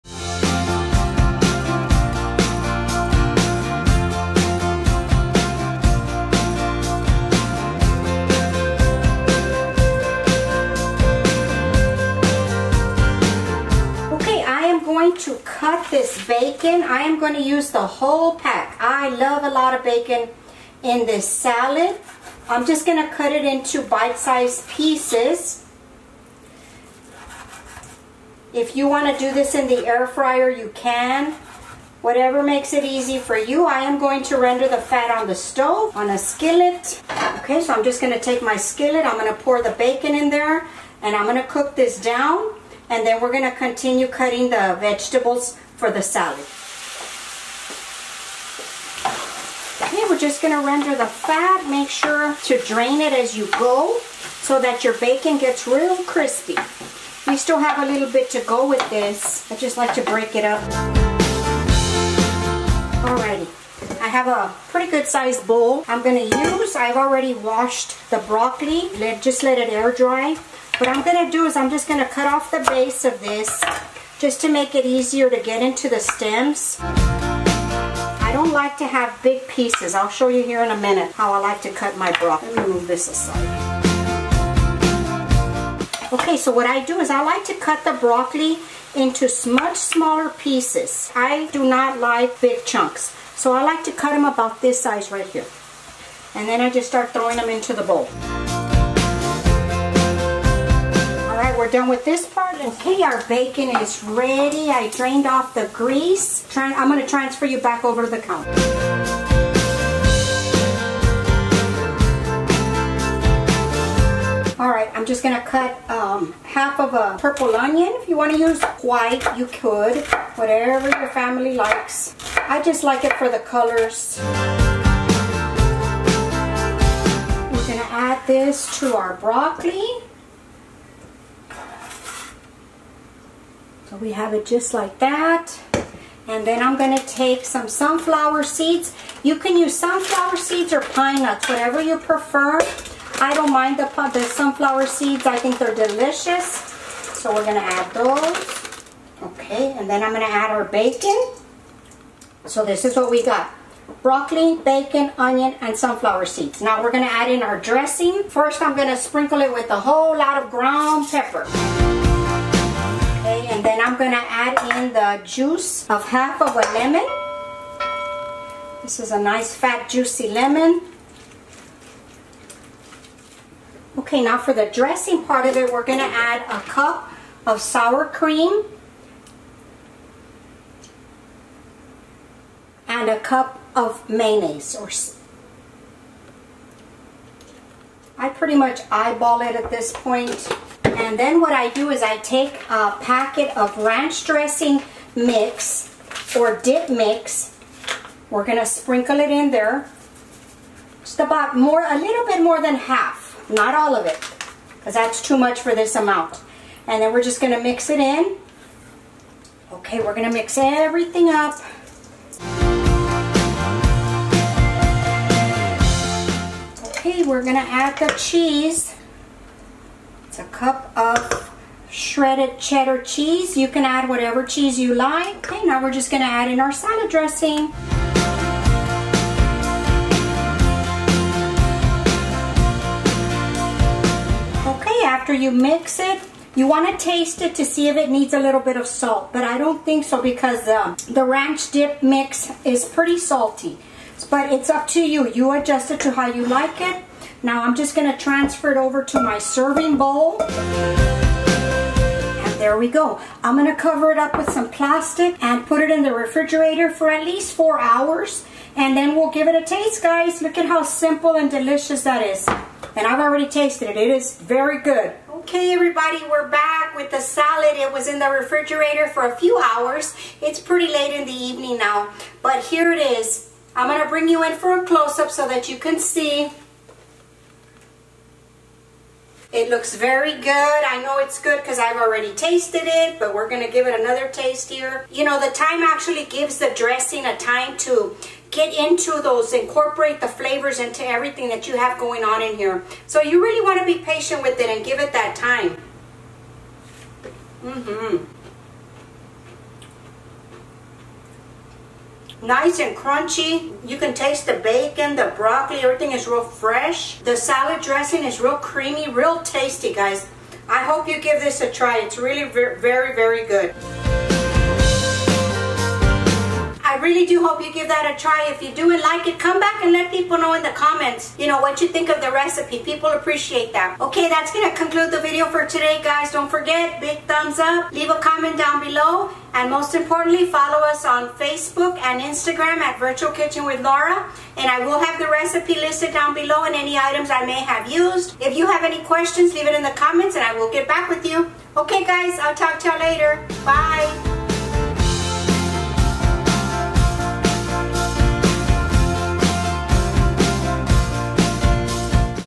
Broccoli Bacon and Cheese Salad ~ Ensalada de Brócoli Tocino y Queso
Hey there, thank you for stopping by. If you are looking for a delicious salad to try out this summer you have come to the right video. This Broccoli, Bacon and Cheese Salad is the perfect thing to accompany and dish. When preparing this Broccoli, Bacon and Cheese Salad make sure to allow time for the salad to sit in the frig for at least 4 hours, overnight is even better.
xoxo
Laura

Links
Lemon rimmer
ranch dressing

Recipe
2 crowns Broccoli
1 lb bacon
1 cup mayonnaise
1 cup of sour cream
1/2 purple onion
1/2 cup sunflowers seeds
1 cup Sharp Cheddar cheese
3/4 packet Ranch diplots of ground pepper
I would not add salt until after you taste it.
adjust to your liking.

✅ Sign up to Rakuten using my link and receive cash back, if you are kind enough to use my link I will also benefit.
✅ Instagram virtualkitchenwithlaura